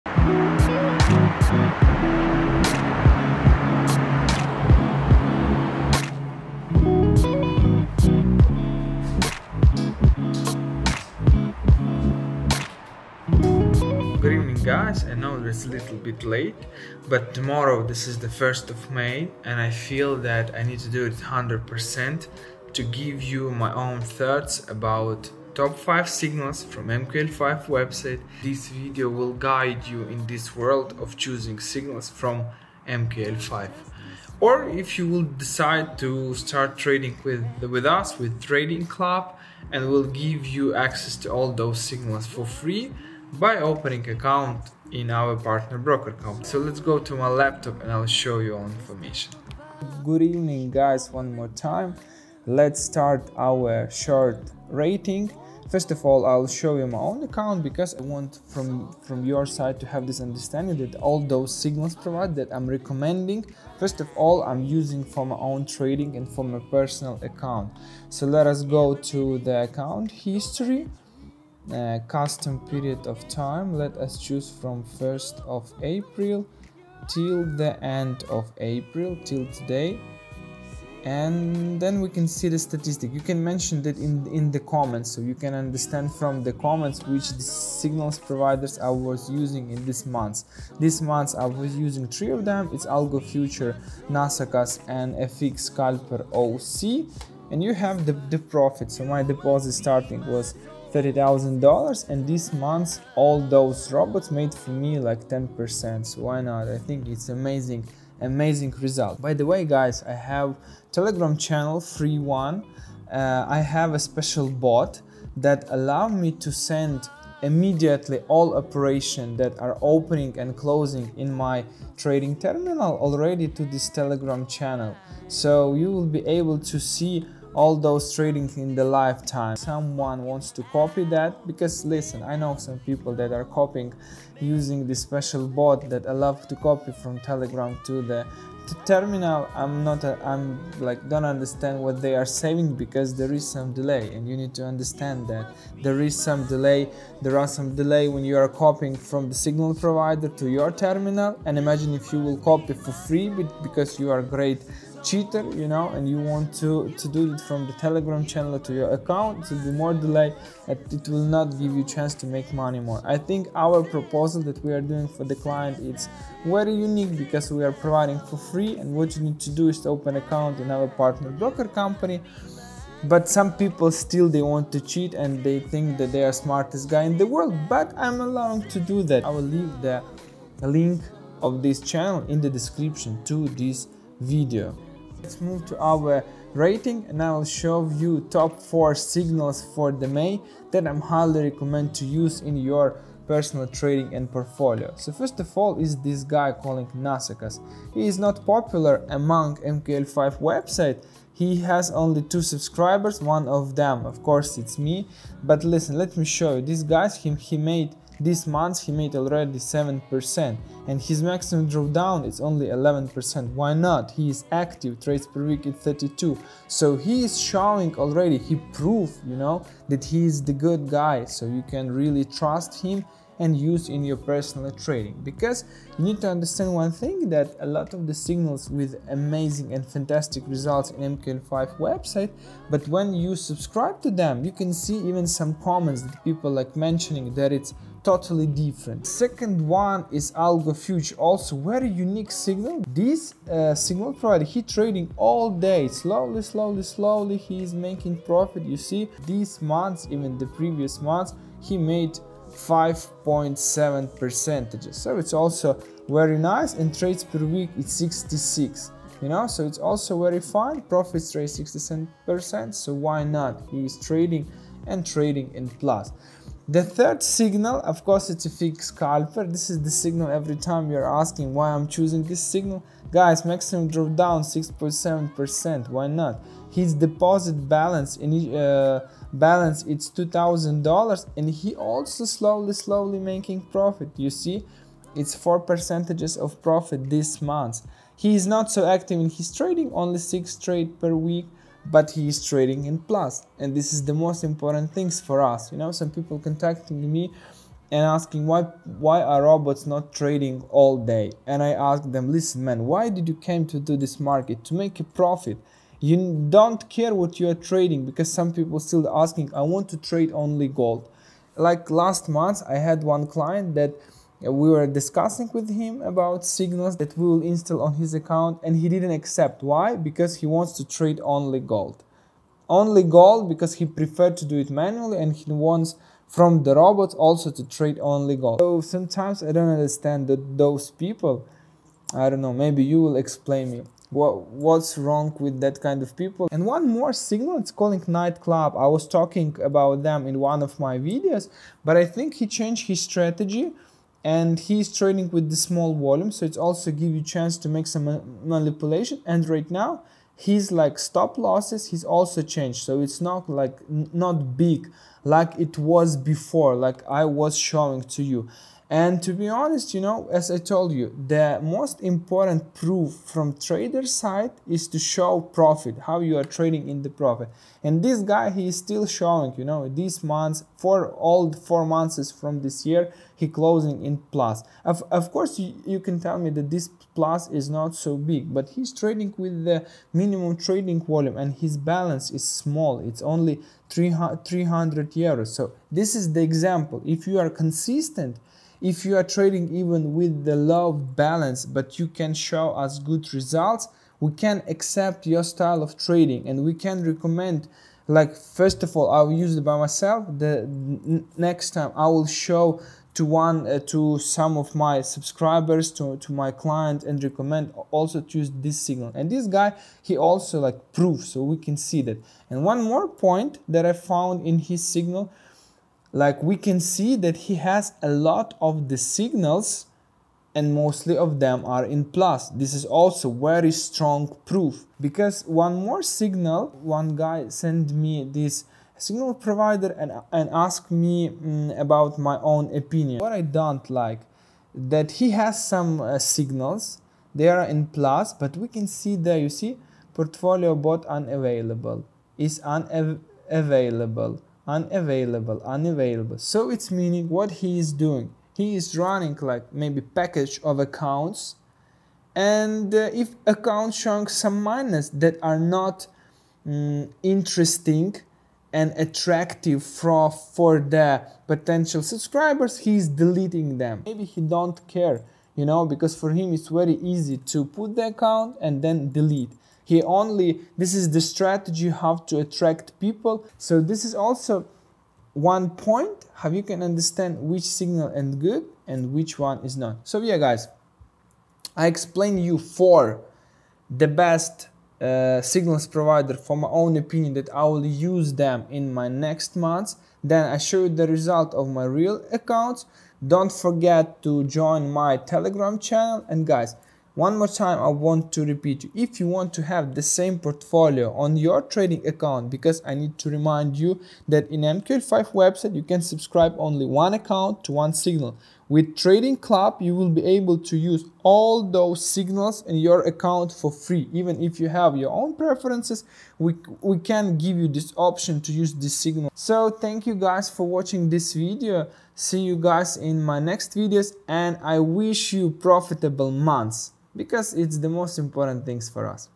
Top 4 best Forex Signals Providers. May 2022. MQL5 signals
Many people all around the world are looking for profitable solutions in trading and investing.
Some of them make a good decisions by using MQL5 signals servise. This is really a great place to find a good money manager for your capital. At the same time it is very difficult to understand which one is good and which one is not.

- Live Streaming all signals direct from Meta Trader4

- message me and I will share with your investor password with live trading of this signals